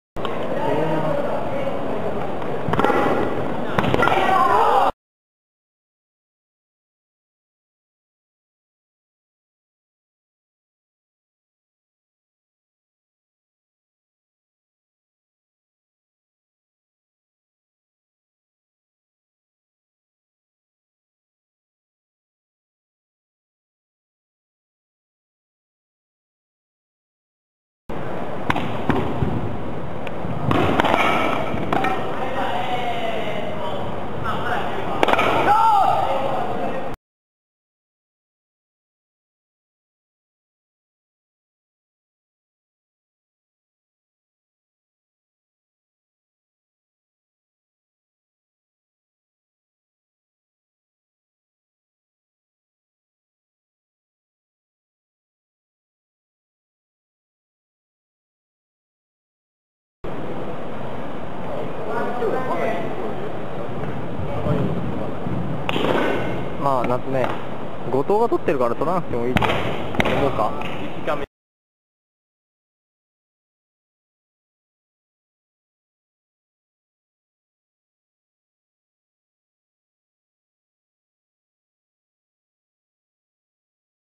有段男子長剣フリー決勝 スポーツチャンバラ関西学生大会2009
原選手（愛媛大）ｖｓ磯上選手（神戸学院大）
This is a video of  ports chanbara championship in japan